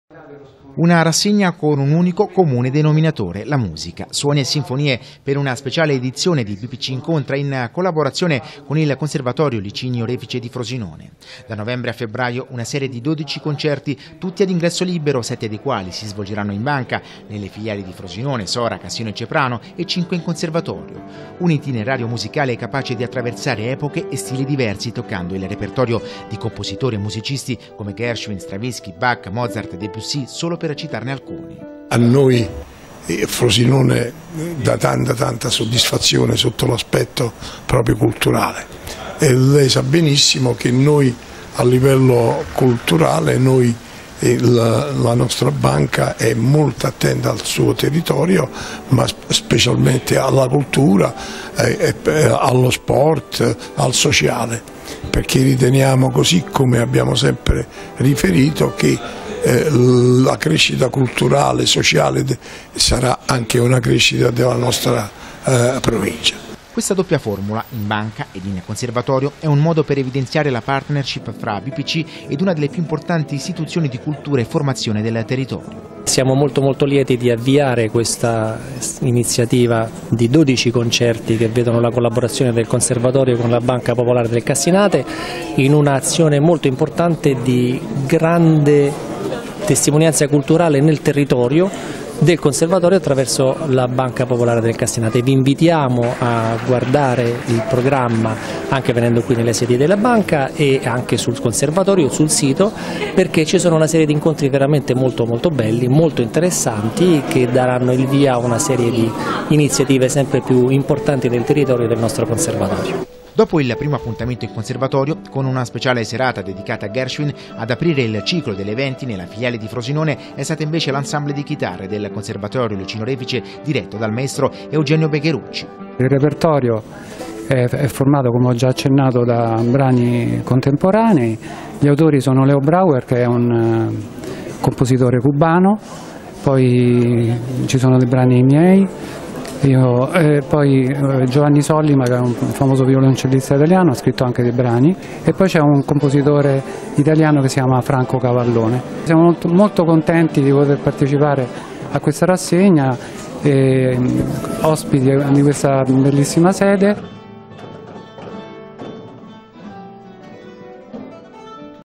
FROSINONE: La BPC incontra la musica del 25-11-2017
Dodici concerti in un percorso musicale che attraversa epoche e compositori con stili diversi. Il tutto all'interno di una speciale edizione di Bpc Incontra in collaborazione con il Conservatorio Licinio Refice di Frosinone.